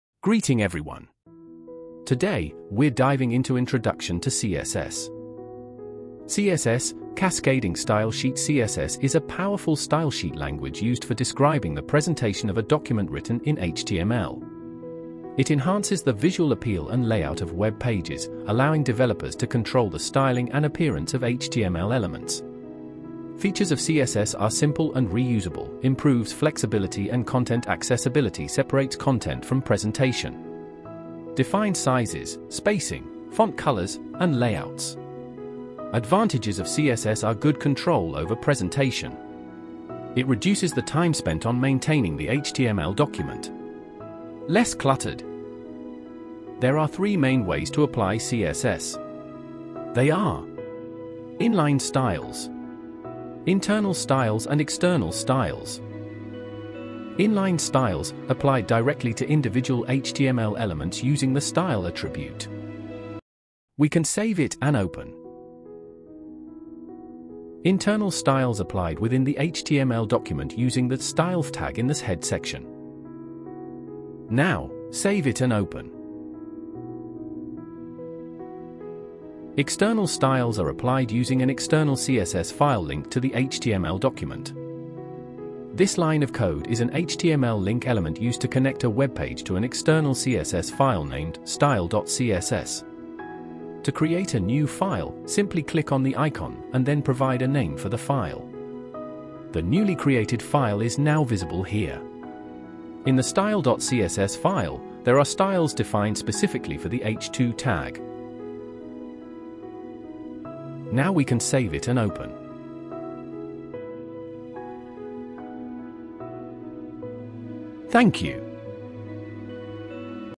Cyber Square Grade 6 Module 2 - Introduction to CSS
Get started with CSS (Cascading Style Sheets) in this beginner-friendly tutorial. Learn the basics of styling web pages, including how to change fonts, colors, layouts, and more using CSS. Whether you're new to web development or looking to enhance your skills, this video will give you a solid foundation in CSS for creating visually appealing and responsive websites.